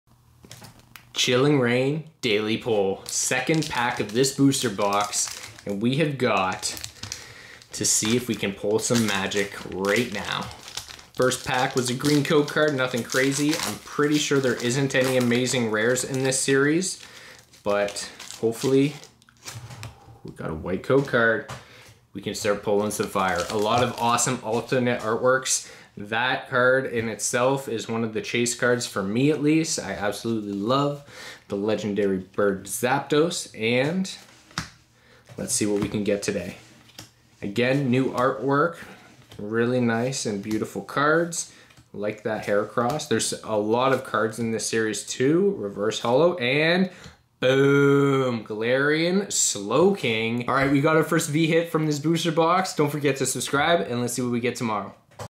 Brand New Chilling Reign Pokémon Pull! Card Opening
Contact through @corruptcosplay on Instagram
Join me on my adventure to collect them all!!!
Here we are opening up multiple Pokémon Card packs ranging from new and old. Always seeking rare cards and hopefully one day a base set first edition Charizard!